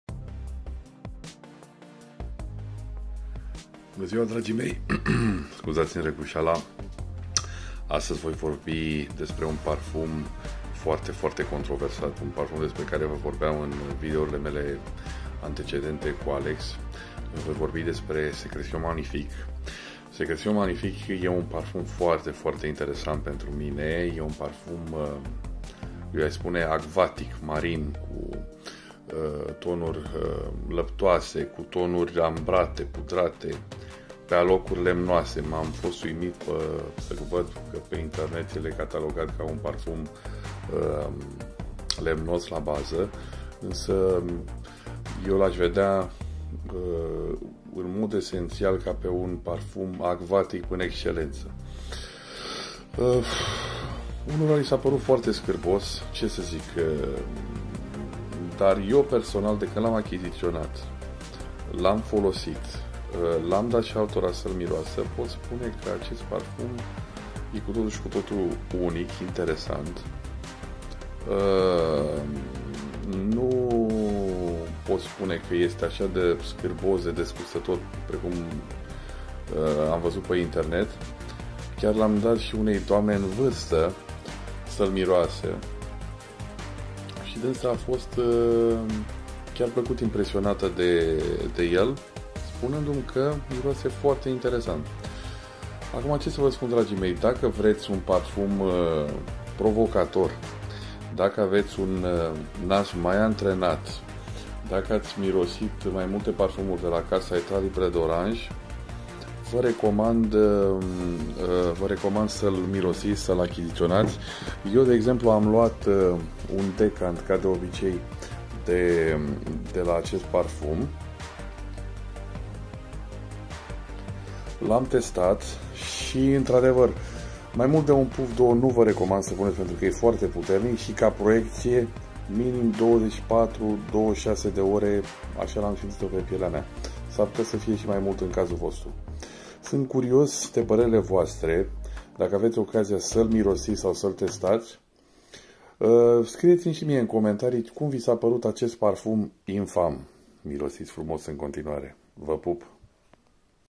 Etait Libre D'Orange - Secretions Magnifique ( Primă Impresie, Recenzie și Concluzii Finale)
Etait Libre D' Orange - Secretions Magnifique

Un parfum extrem de controversat, detestat de mulți, iubit de alții. Antoine Lie, nasul din spatele acestui parfum a reușit să recreeze, prin folosirea unor acorduri de sânge, spermă, salivă și transpirație ,,aroma", dacă vreți, a unei partide de sex sălbătic, lipsit de compromisuri.

Eu am testat acest parfum...performanța este de-a dreptul colosală, siajul bun în primele ore.

Un parfum lemnos, cu note acvatice, marine, lactonice și lemnoase excelent, asta în umila mea opinie, doar...

Sunt extrem de curios și de părerile voastre. Lăsați-mi un comentariu și spuneți-mi ce impresie v-a făcut cel mai ,, detestat" parfum din toate timpurile! ☺️